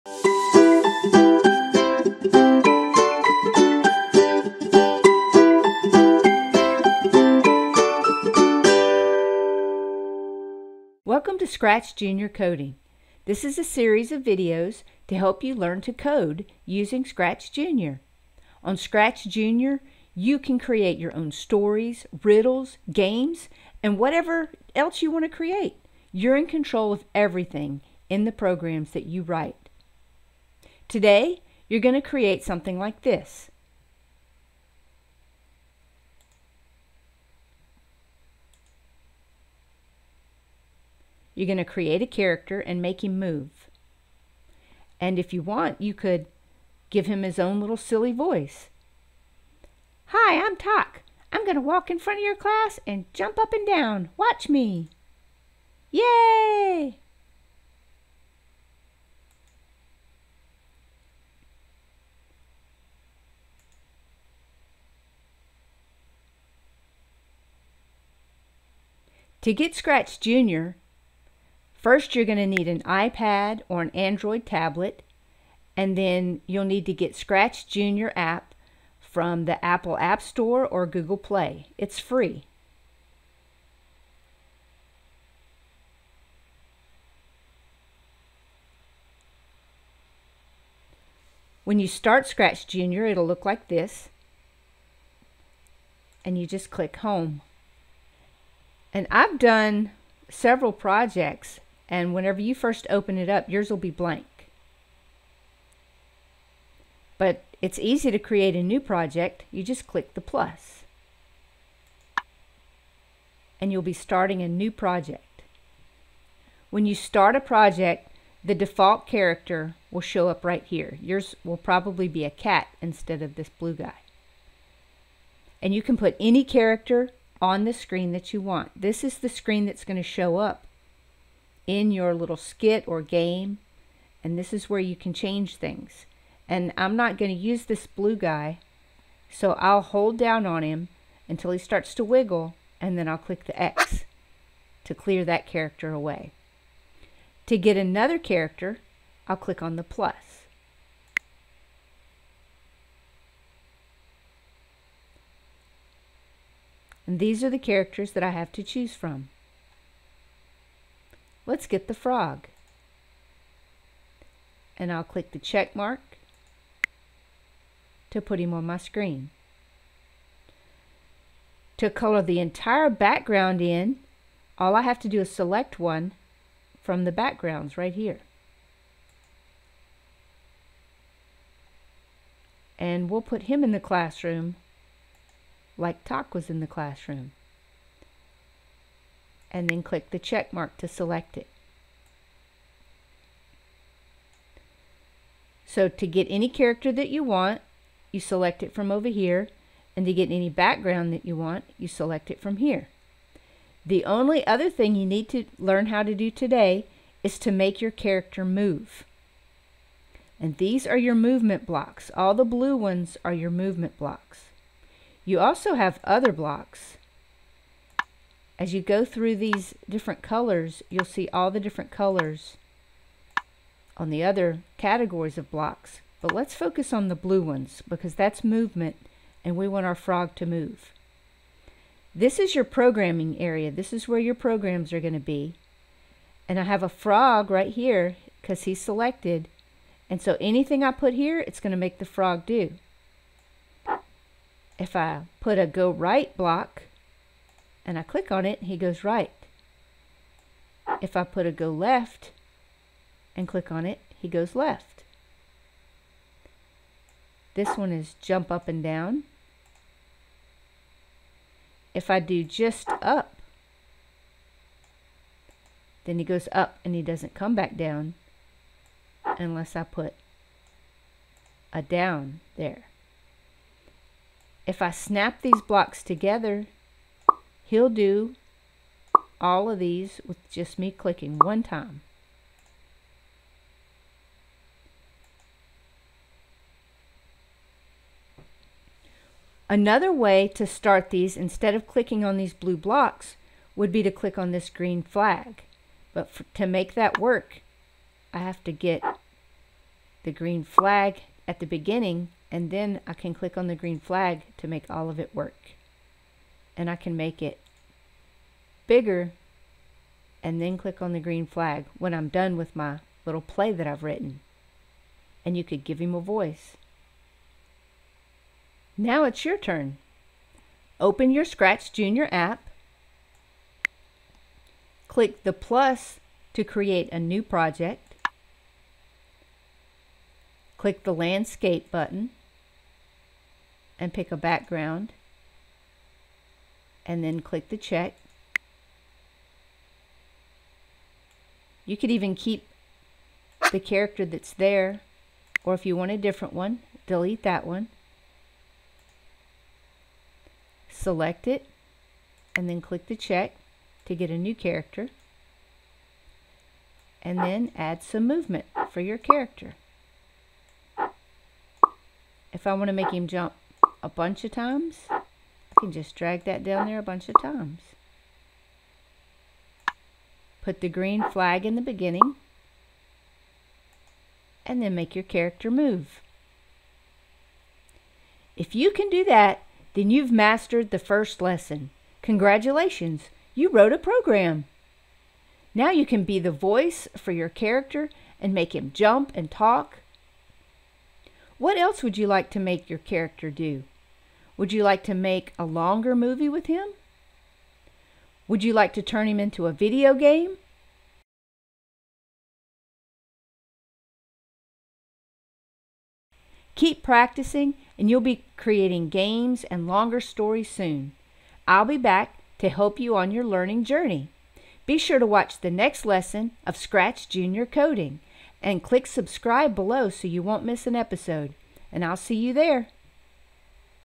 ScratchJr Coding Lesson 1 | How to Make Character Move | Free Programming Lesson1
Free kids coding lessons to teach beginner computer programming concepts. This 15 lesson ScratchJr video series helps teachers introduce coding to 5-8 year olds using the ScratchJr app. Lessons build on each other as new skills are learned.
Fun for anyone, but great for kindergarten through second grade.
Good for students on their own, in the classroom, or in homeschool. Simply watch the video, then code what you saw.

Lesson 1 introduces the app and helps the student write their first program which makes the character move in the classroom.